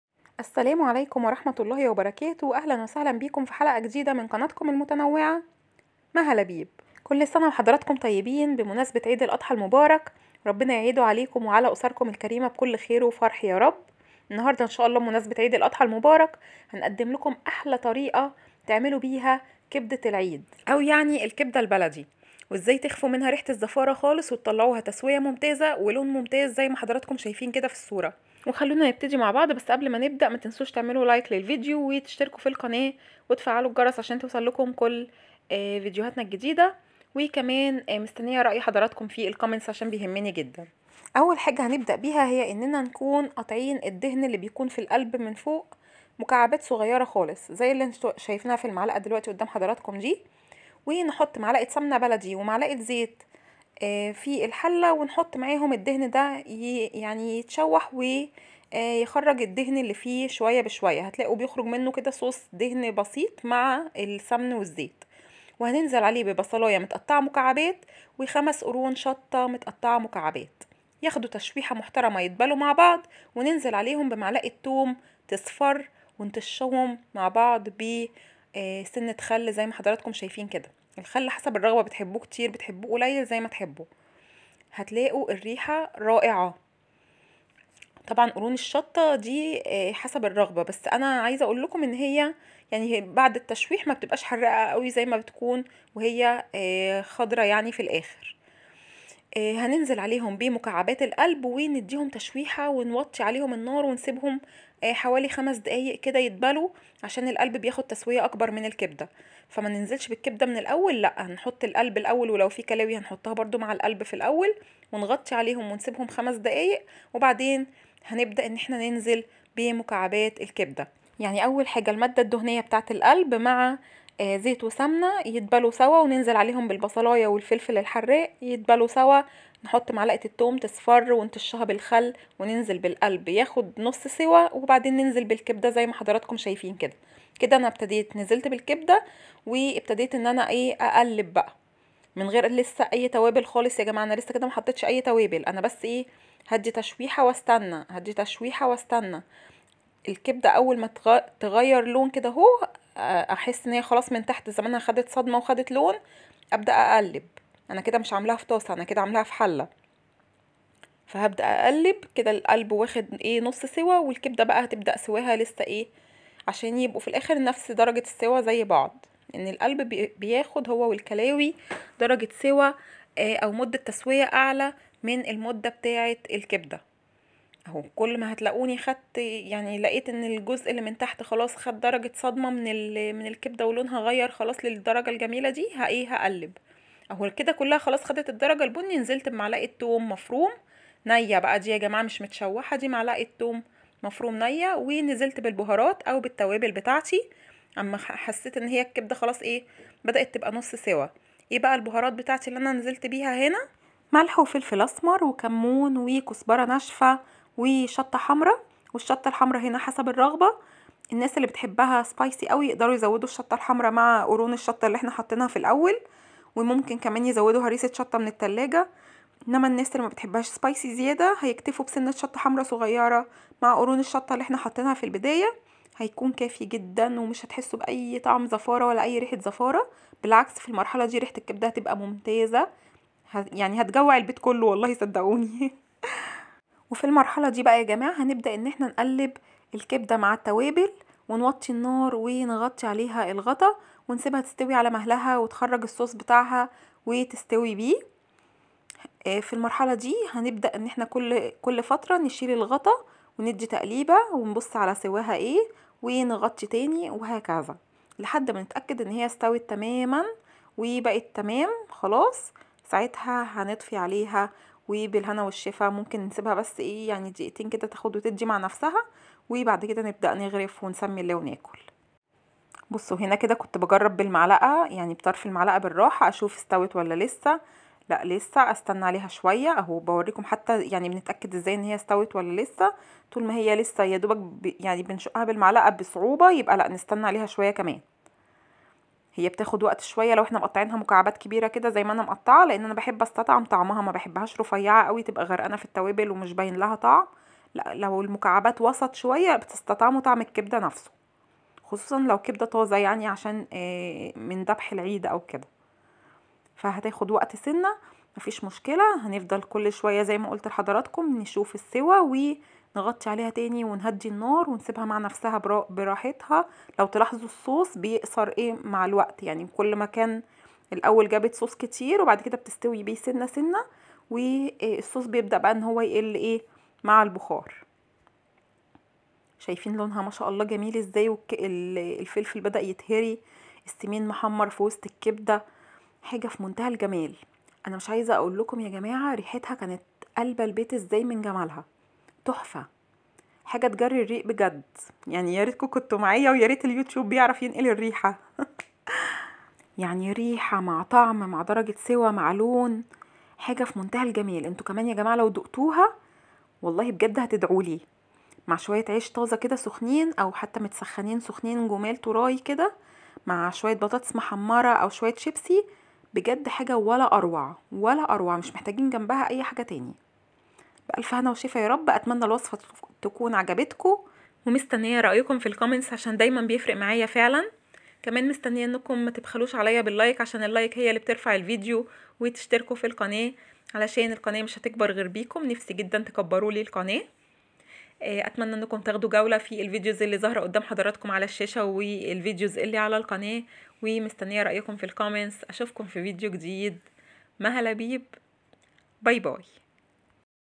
أحلي طريقة للكبدة البلدي/ كبدة العيد كما يجب أن تكون/ جربيها وهتدعيلي/ كل سنة وانتم طيبين/Maha Labib
أجمل طعم ورائحة ولون وتسوية هتشوفيها لكبدة العيد أو الكبدة البلدي